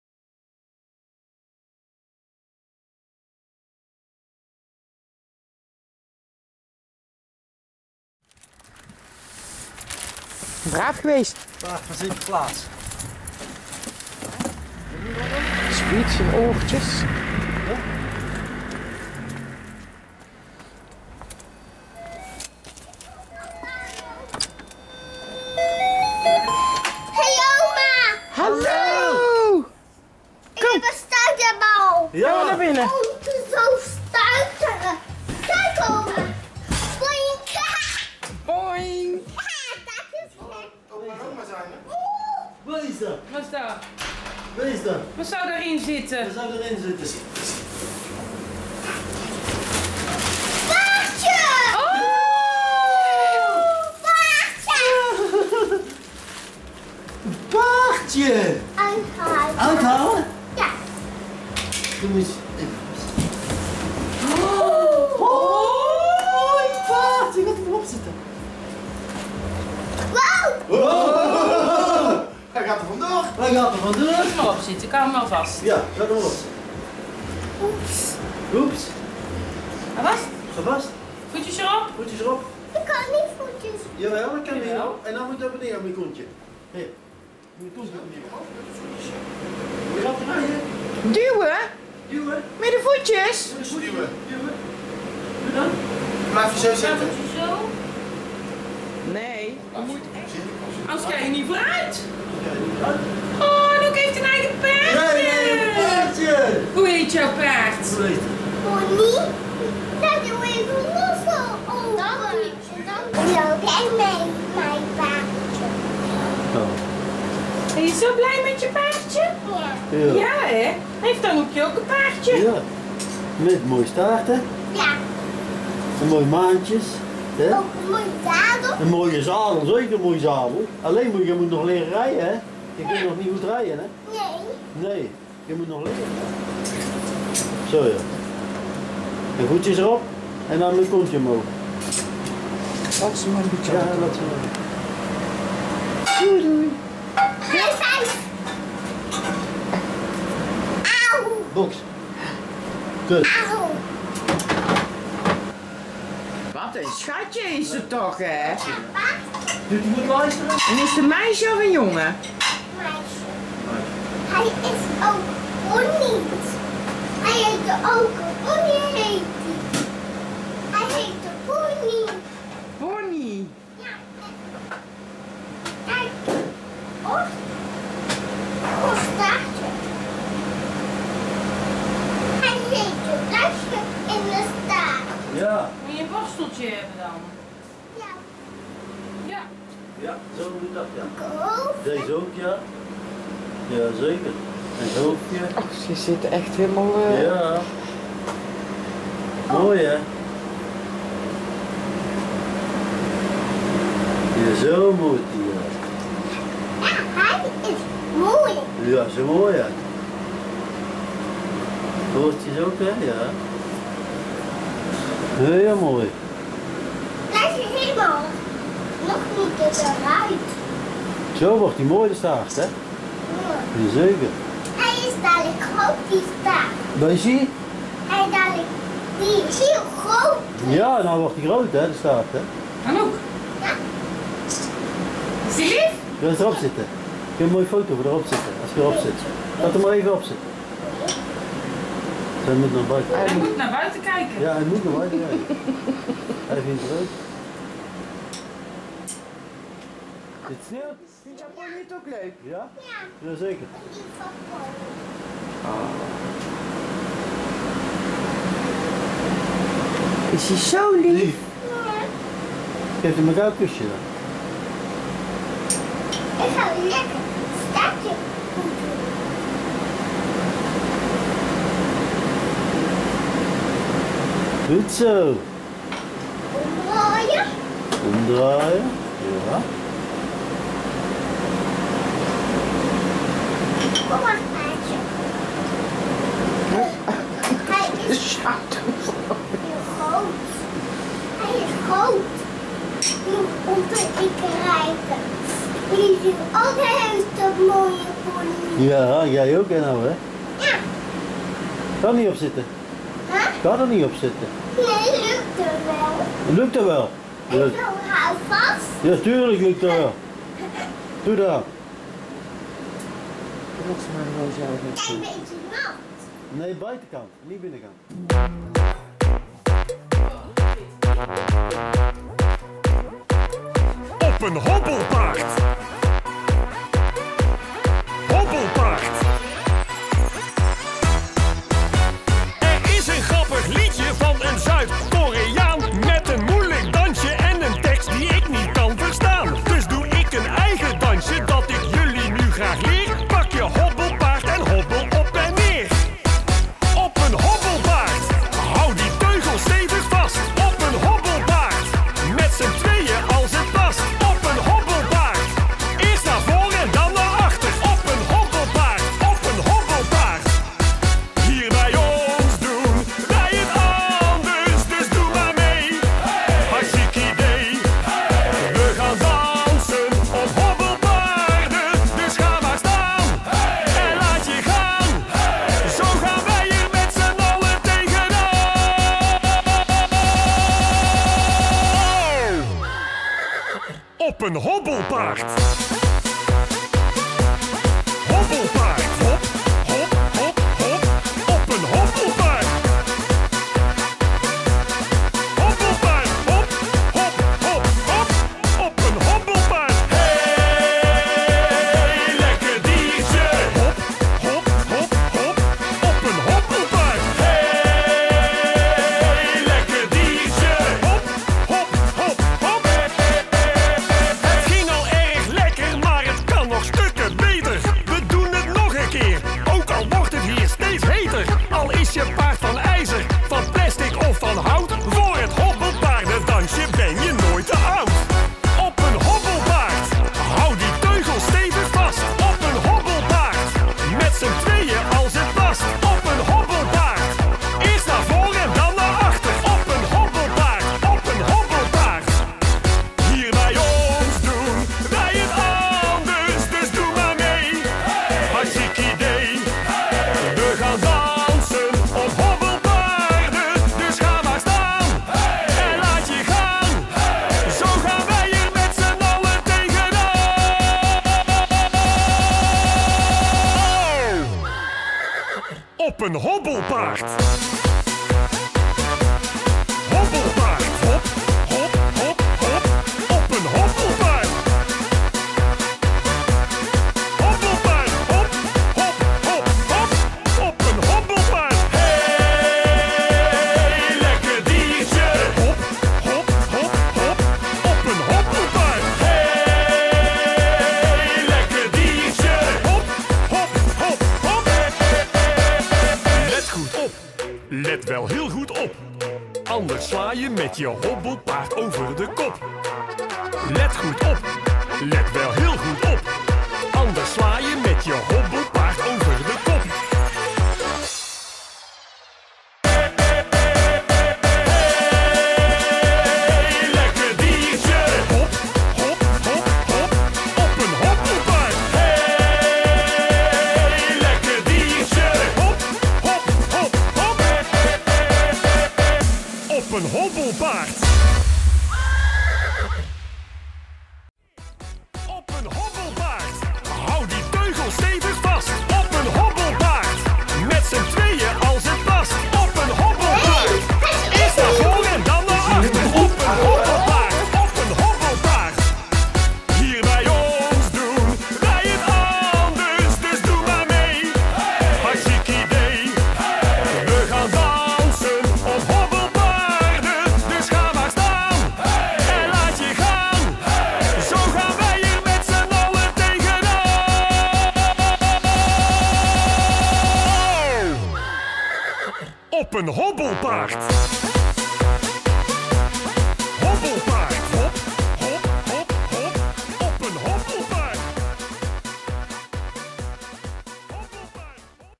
Anoekje ook een eigen pony
Anoekje wou ook graag een eigen pony, net als haar zusje Lizzy.
Dus die gingen we maar even brengen vandaag.
Helemaal happy met haar eigen pony die anoek